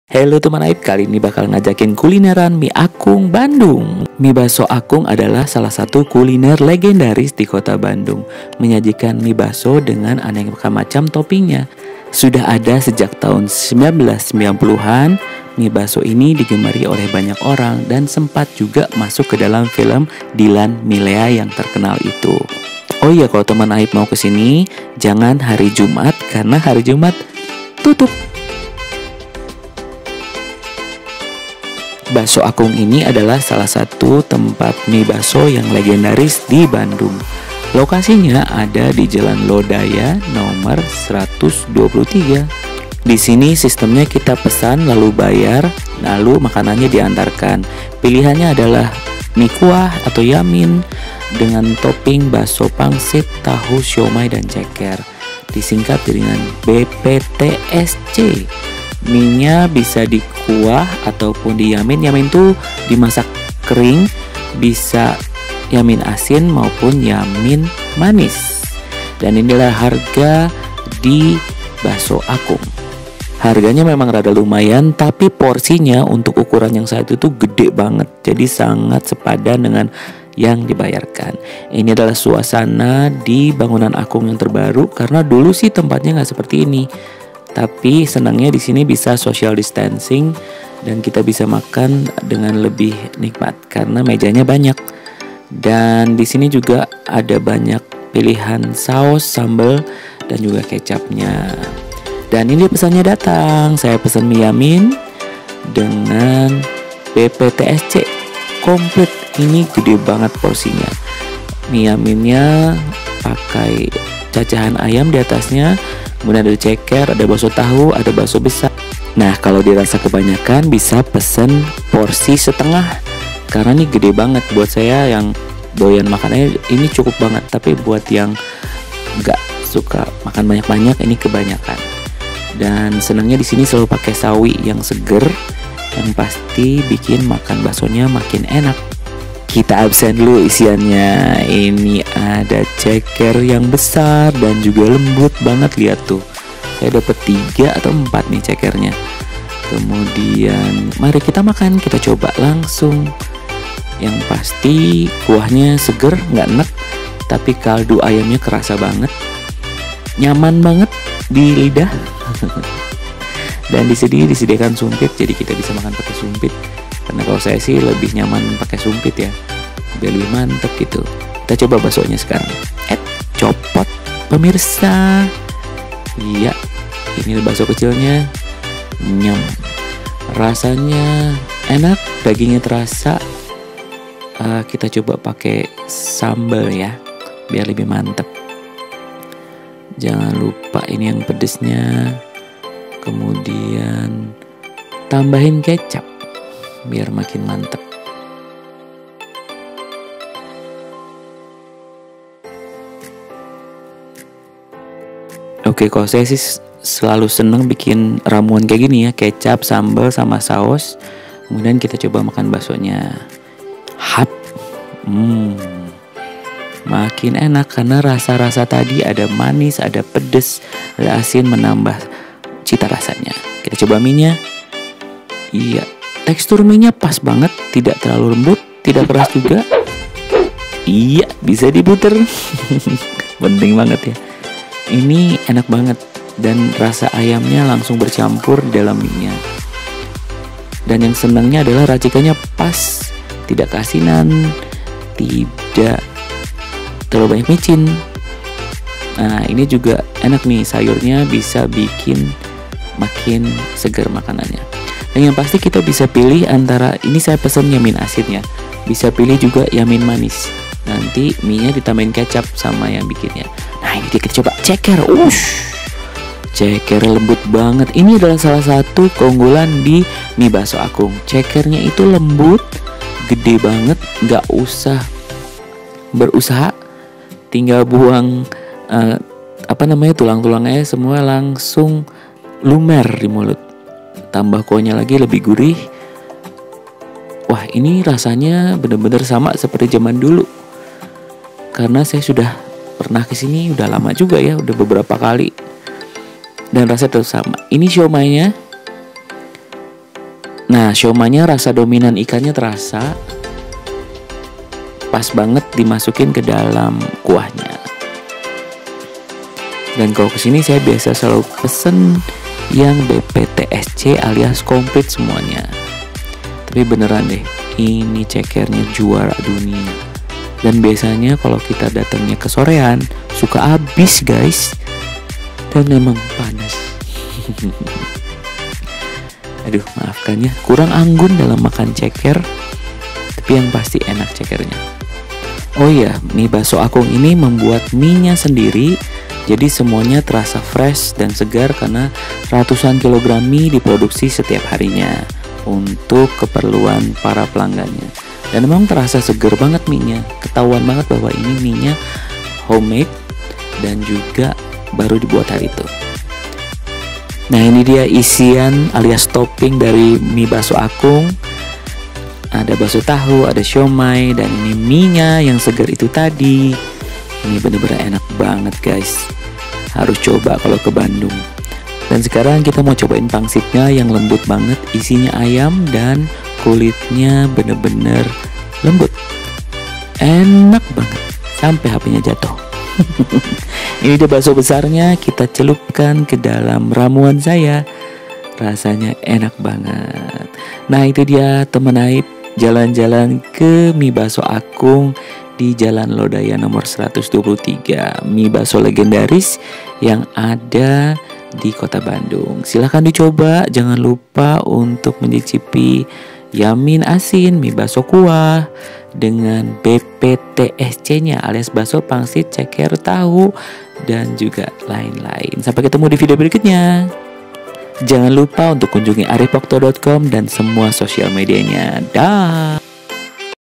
Kuliner Bandung Mie Bakso Akung Bandung
mencicipi Mie Bakso Akung Bandung yang legendaris dengan Yamin BPTSC alias Bakso Pangsit Tahu Siomay Ceker.